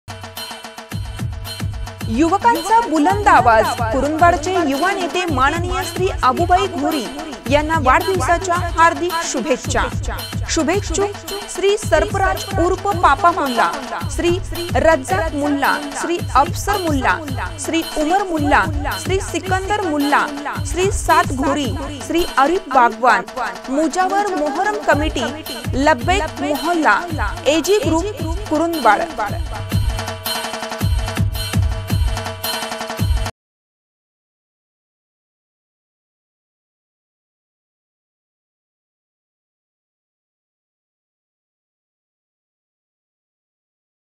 ADVT- श्री.अबूभाई घोरी यांना वाढदिवसाच्या हार्दिक शुभेच्छा...!
ADVT- युवकांचा बुलंद आवाज, कुरुंदवाडचे युवा नेते माननीय श्री. अबूभाई घोरी यांना वाढदिवसाच्या हार्दिक शुभेच्छा...!
-: शुभेच्छुक :-
श्री. सरफराज ऊर्फ पापा मुल्ला, श्री. रझ्झाक मुल्ला, श्री. अफसर मुल्ला, श्री. उमर मुल्ला, श्री. सिकंदर मुल्ला, श्री. साद घोरी, श्री. आरिफ बागवान
मुजावर मोहरम कमिटी
लब्बैक मोहल्ला
ए. जी. ग्रुप, कुरूंदवाड